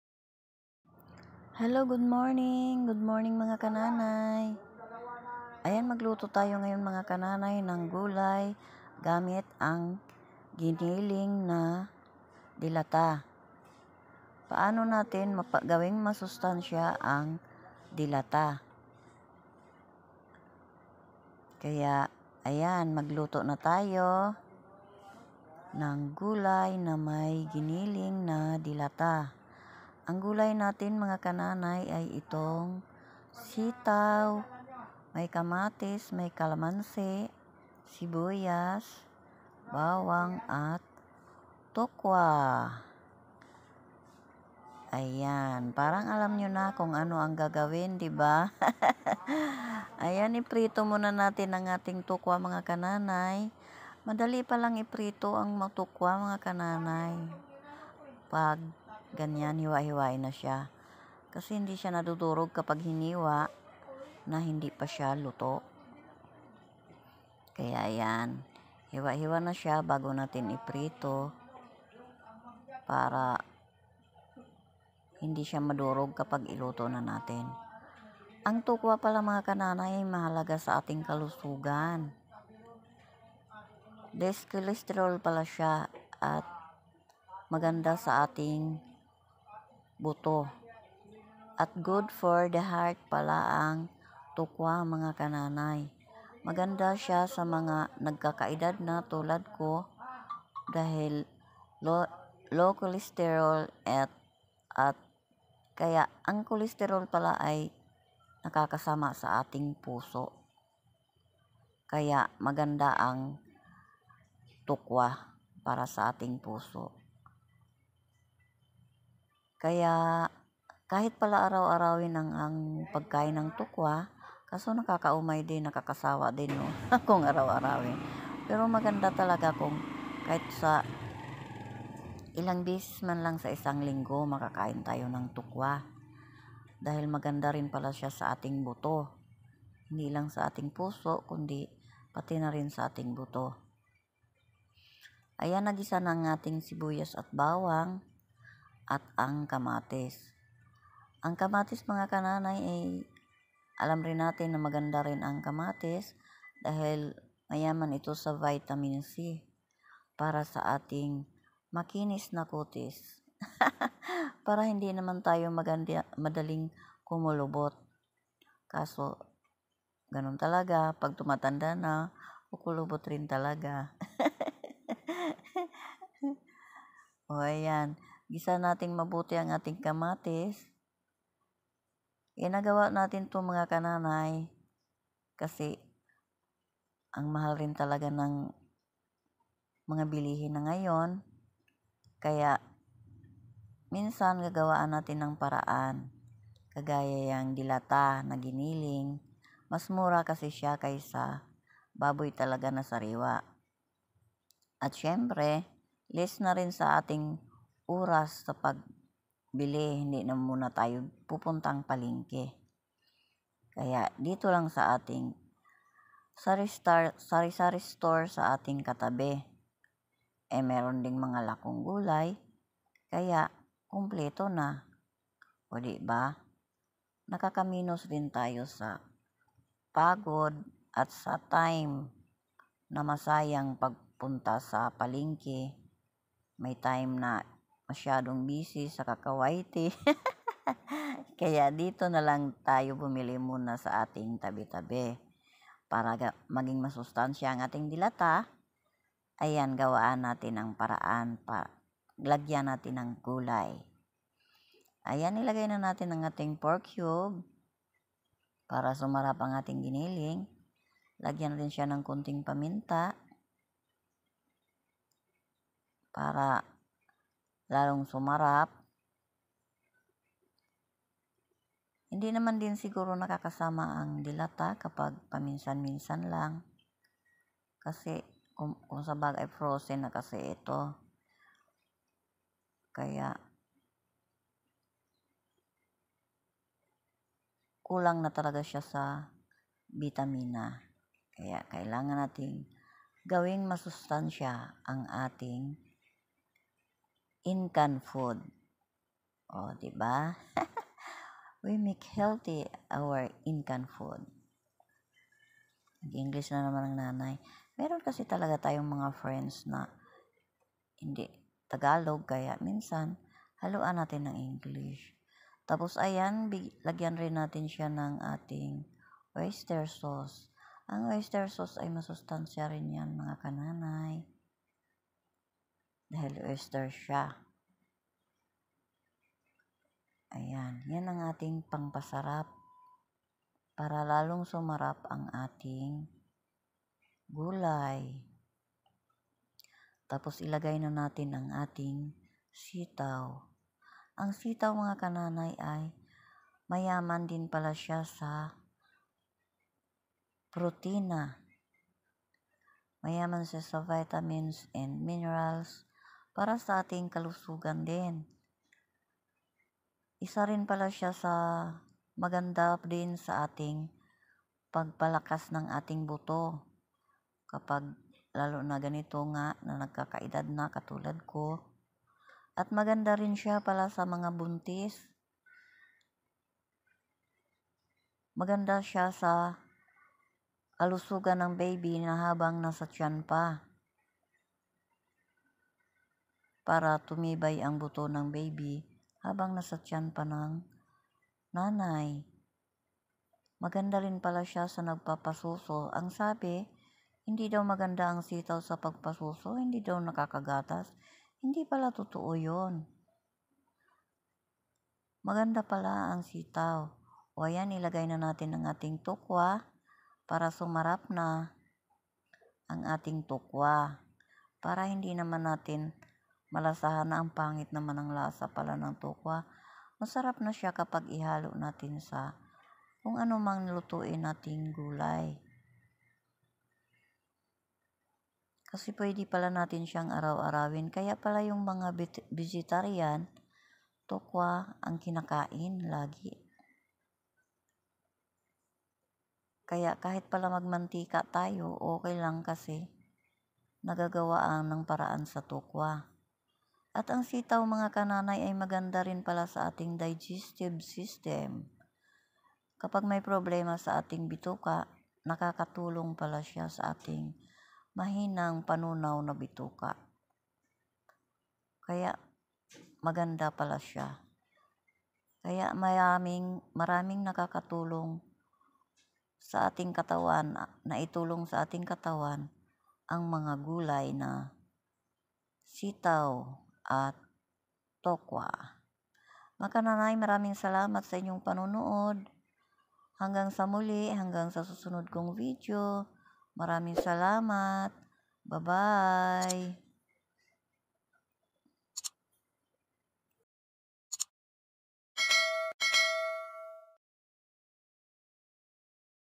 Paano gawing Healthy ang delatang giniling sa pamagitan ng Gulay na Sitaw &Tufo
Paano gawing healthy ang delata sa pamagitan ng gulay na sitaw at tukwa ,itoy ating lutoing masarap na gulay para sa ating kalusogan ,para pampahaba ng ating buhay